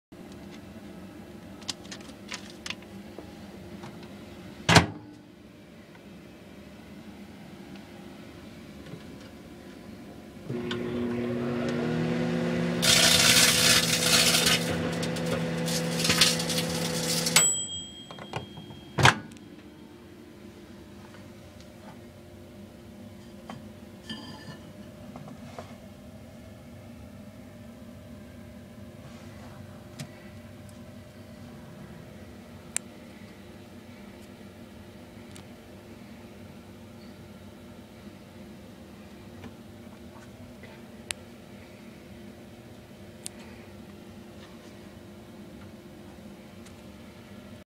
How To Shrink a Bag of Chips
This is how to shrink a bag of chips in the microwave. Just empty the bag, flatten the bag, and put in the microwave for 5 seconds or longer for bigger bags. I won't be responsible if your microwave gets damaged from this awesome science experiment. NO BAGS OF CHIPS WERE SEVERELY HARMED IN THE MAKING OF THIS VIDEO! Write in the comments what you think!